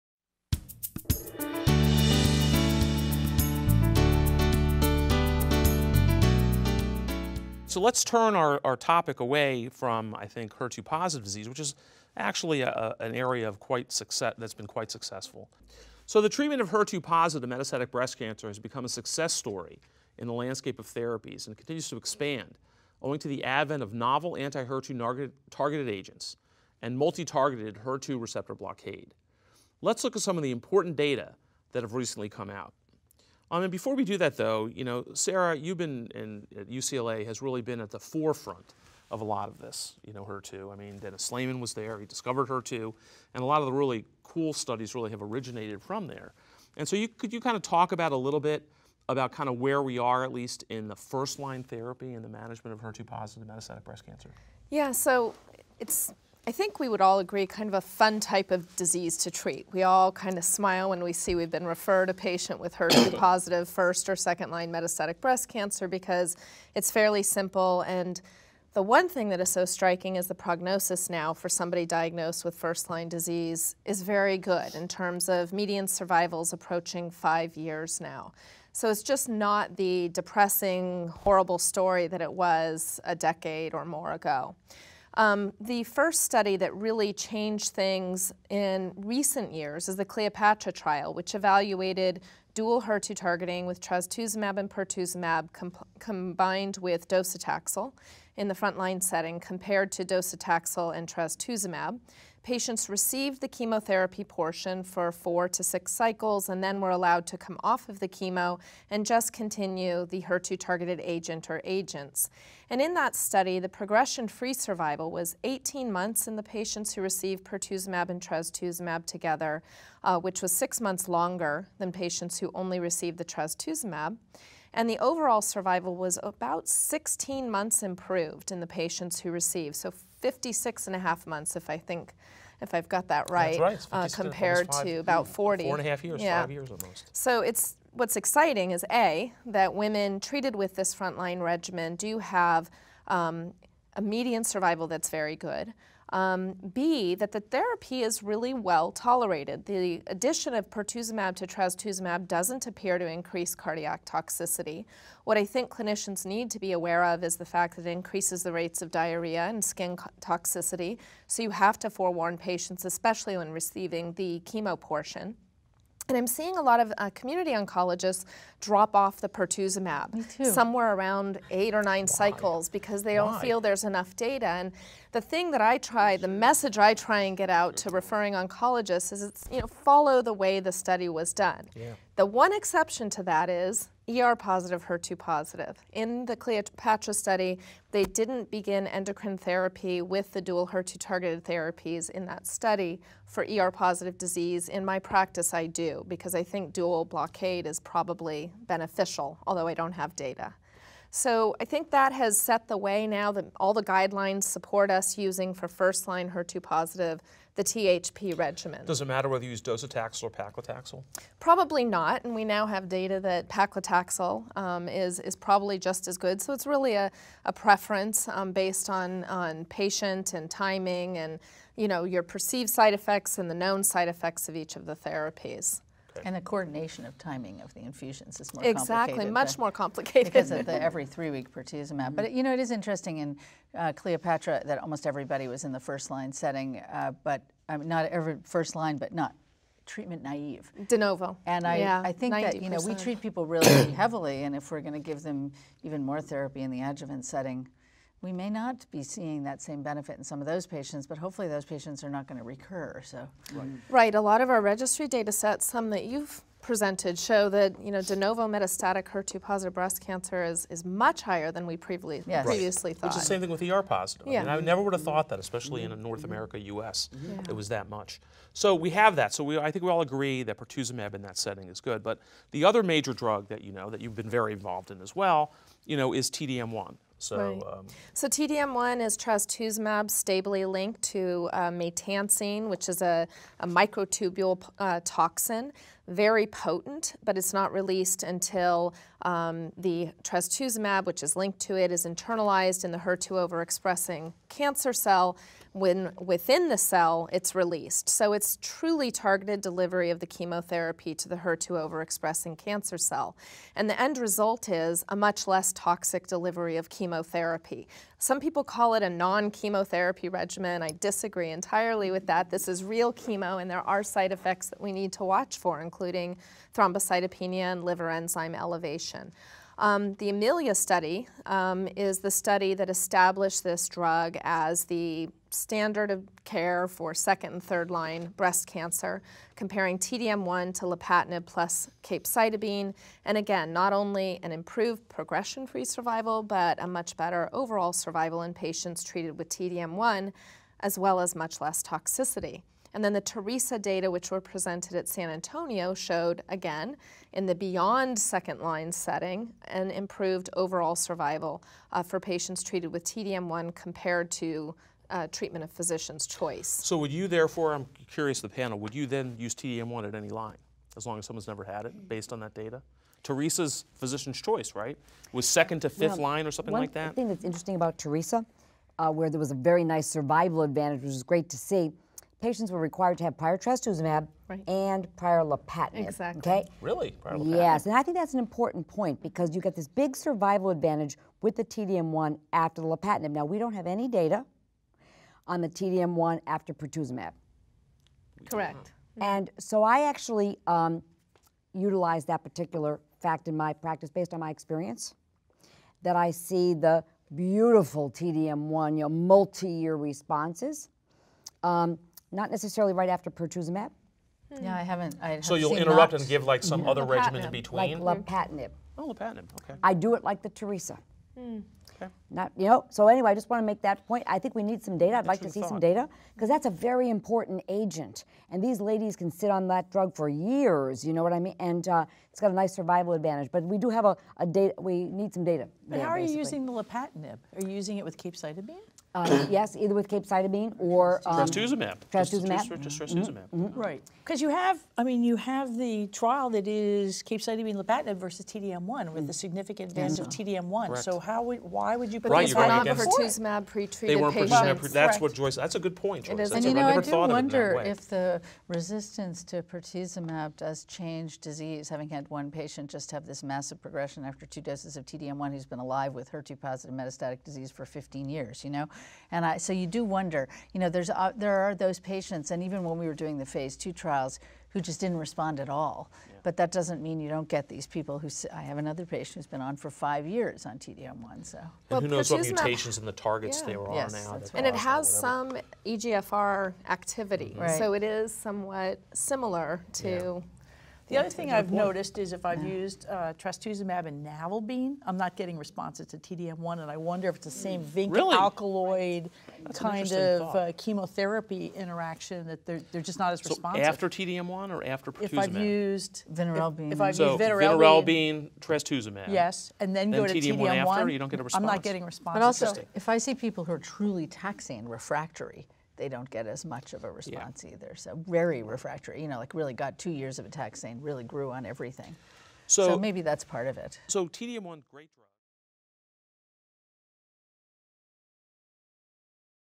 First-Line Treatment of HER2-Positive Metastatic Breast Cancer
The panelists, Adam M. Brufsky, MD; Sara A. Hurvitz, MD; Hope S. Rugo, MD; Joyce A. O’Shaughnessy, MD; and Christy A. Russell, MD, outline first-line treatment options in HER2-positive metastatic breast cancer.